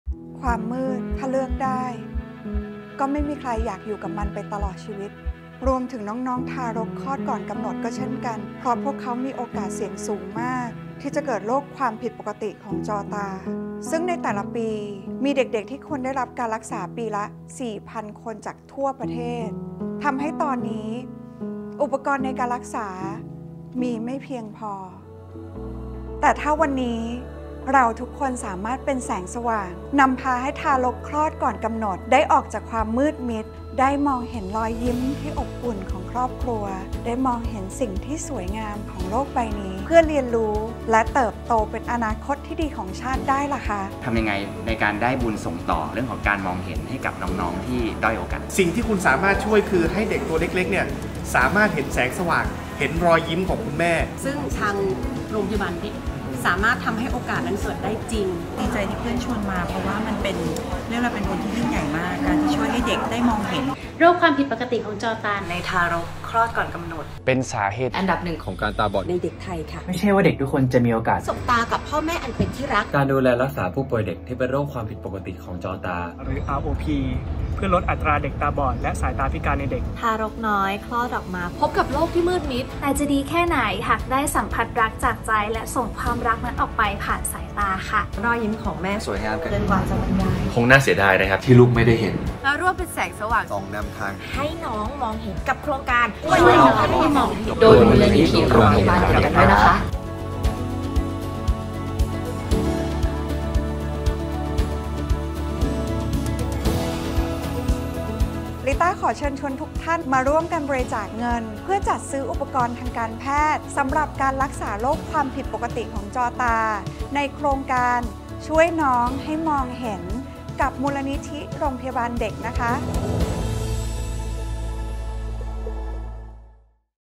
ช่วยน้องให้มองเห็น รวมศิลปินดาราผู้ประกาศร่วมประชาสัมพันธ์
อาจไม่ใช่ทารกทุกคนที่จะมีโอกาสได้มองหน้า สบตากับพ่อแม่อันเป็นที่รัก เพราะเด็กบางคนเกิดมาพร้อมกับโรคจอตาผิดปกติ

🩷โรคความผิดปกติของจอตา ในทารกคลอดก่อนกำหนด เป็นสาเหตุอันดับหนึ่ง ของการตาบอดในเด็กไทย  อยากชวนทุกคนมาช่วยกันเพิ่มโอกาสการรักษาผู้ป่วยเด็กที่เป็นโรคความผิดปกติของจอตา หรือ ROP เพื่อลดอัตราเด็กตาบอดและสายตาพิการในเด็กไปด้วยกัน
♥️ ร่วมสร้างปาฏิหาริย์ เป็นแสงสว่างส่องนำทาง ให้น้องมองเห็น กับโครงการ
•♥️บัญชีมูลนิธิโรงพยาบาลเด็ก ธนาคารทหารไทยธนชาต TTB เลขบัญชี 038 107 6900
•👉หรือบริจาคง่ายๆทางเว็บไซต์ที่
(Edonationลดหย่อนภาษี 2 เท่า)   Line@Givetochild